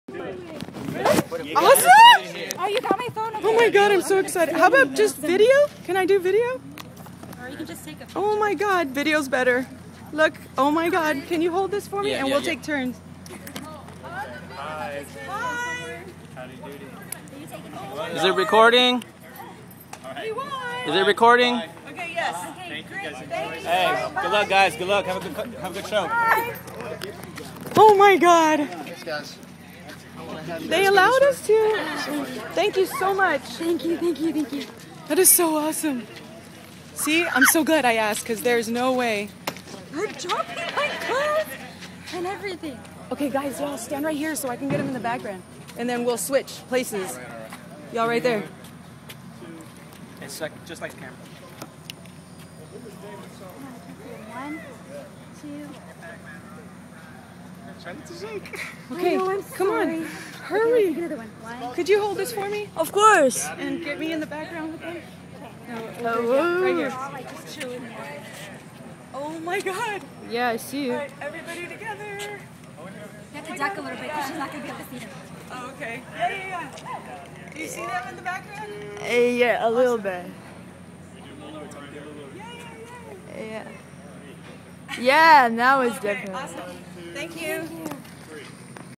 Jared Leto live meeting backstage
we got VIP meet and greet at concert with 30 sec to Mars this was front stage
Jessica and JaeRae